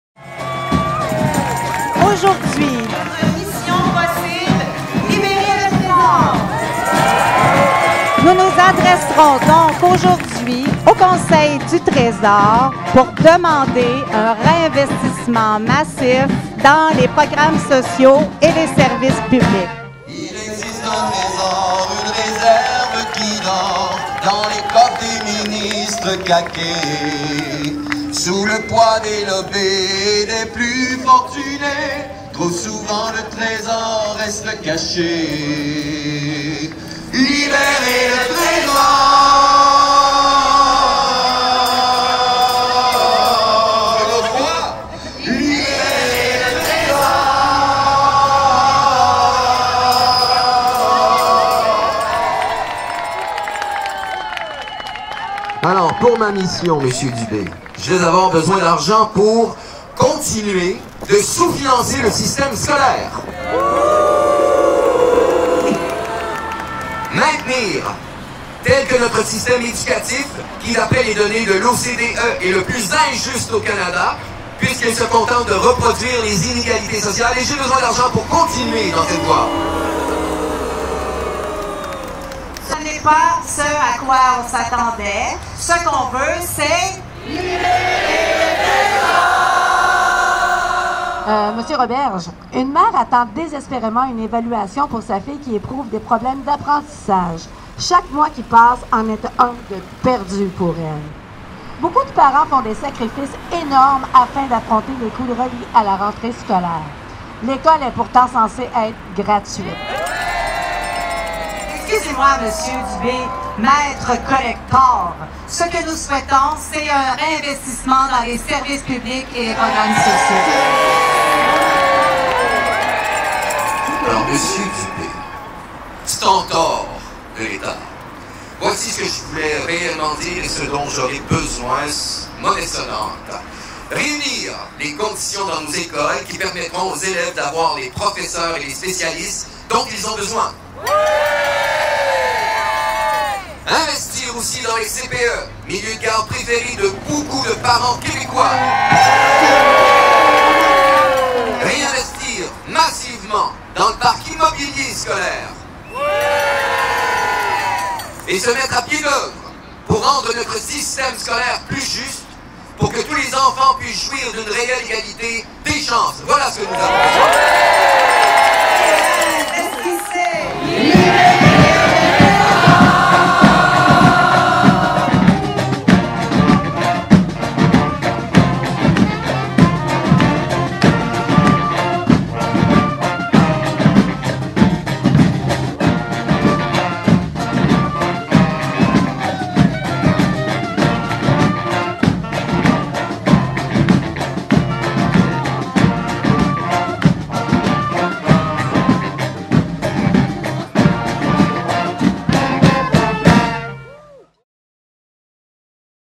[Budget 2020] Education: Not enough investments!
Our demands have not been heard!

Montréal community organizations have made demands to the Treasury Board for a massive reinvestment in the 2020 budget. These demands are still relevant!

On October 23, 2019, over 300 people rallied in front of Hôtel-Dieu in Montréal in order to send a clear message to the President of the Treasury Board, Christian Dubé, that he must unleash our treasure and massively reinvest in much-needed public services and social programs.
In fact, over the years, we have been told that we have to tighten our belts while a surplus of funds continues to grow. All the while, this surplus is not used to justly serve the population, especially those who are most in need and experience marginalization. It's unacceptable!

As a group, we called on the following ministers
• du Travail, de l’Emploi et de la Solidarité sociale (Work, Employment, and Social Solidarity),
• de la Santé et des Services sociaux (Health and Social Services),
• de l’Éducation et de l’Enseignement supérieur (Education and Higher Education) and
• de l’Immigration, de la Francisation et de l’Intégration (Immigration, Francization, and Integration)

We created four short videos in order to clearly present our demands to the President of the Treasury Board, Christian Dubé, and to each minister. This video features the Minister of Education, Jean-François Roberge.

We're not giving up!

French audio with English subtitles.
Consultez notre liste de lecture: "[Budget 2020] Libérez le trésor!" pour trouver cette capsule avec des sous-titres en français.